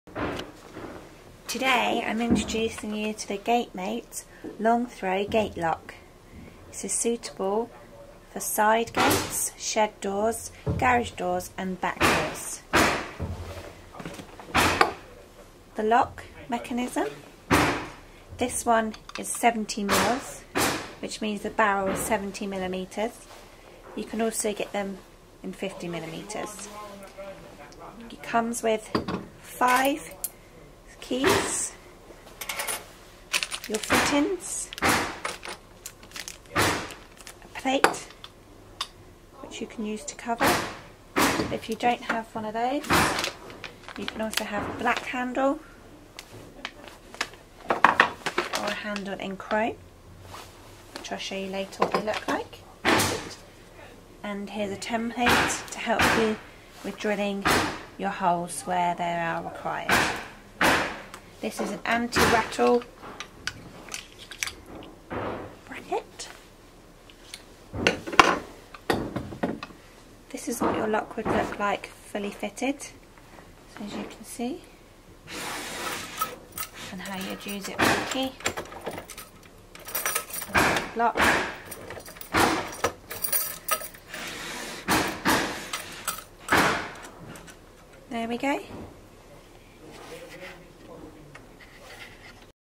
GATEMATE Long Throw Gate Lock
Introducing the Gate Mate Long Throw Gate Lock
* Comes with 5 keys as standard
* Elongated anti-rattle keep
* Chrome escutcheon plate
* High security 6 pin lock barrel
* High security stainless steel screws and driver bit included
* High corrosion resistance; 316 stainless steel locking bolt
* Unique bolt design with hardened core to prevent cutting